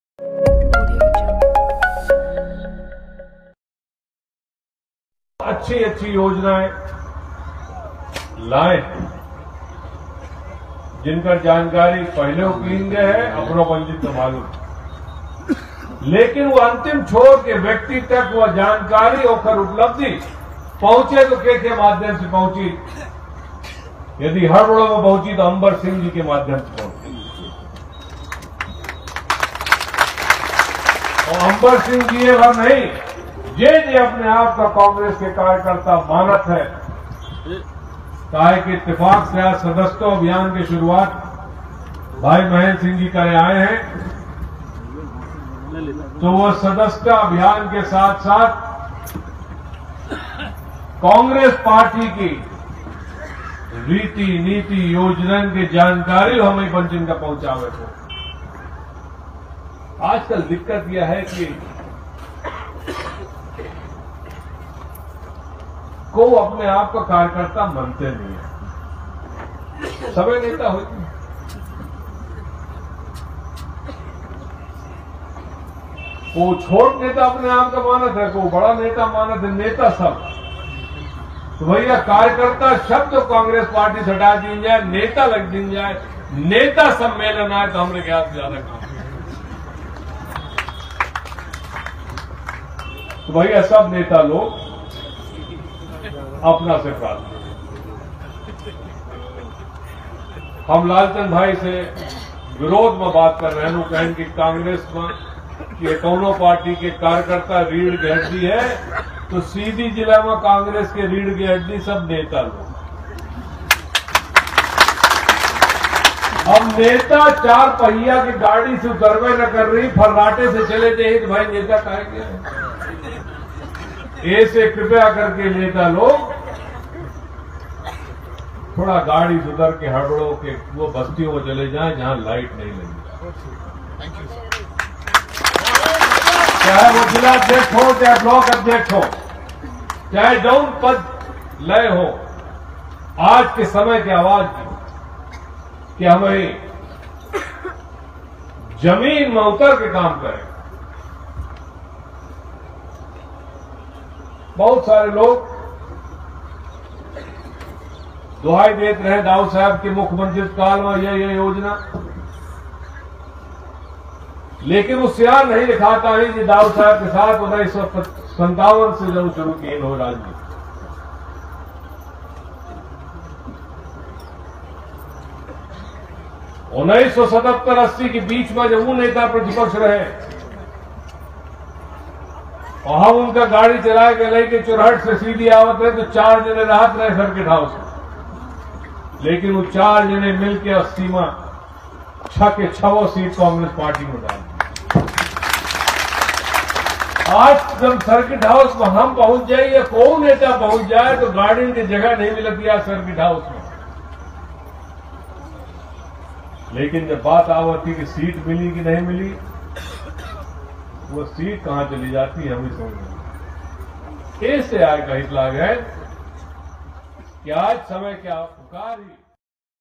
अजयसिंह राहुलसीधी के हडबड़ों में दिया जोरदार भाषण मंच से ही कांग्रेस के पदाधिकारियों कोदी बड़ीनशीहत!
अजयसिंह राहुल:सीधी के हडबड़ों में दिया जोरदार भाषण मंच से ही कांग्रेस के पदाधिकारियों को लगाई फटकार!